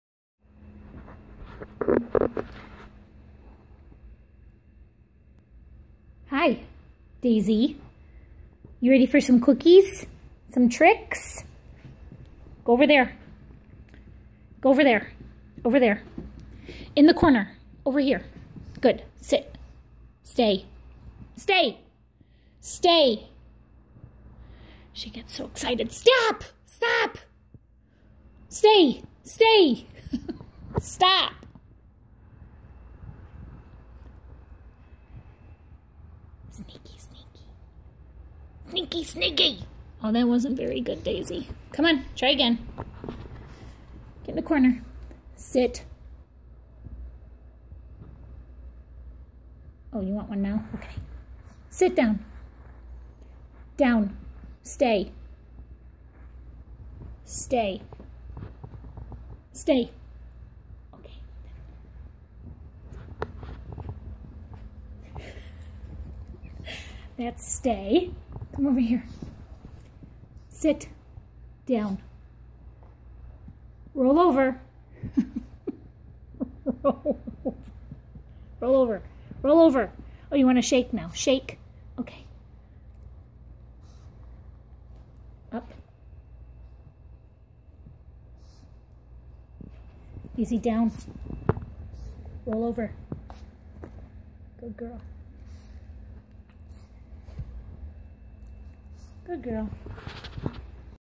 Daisy's tricks
She gets so damn excited to eat the damn treats she can't follow instructions. Lol. Cracks me up.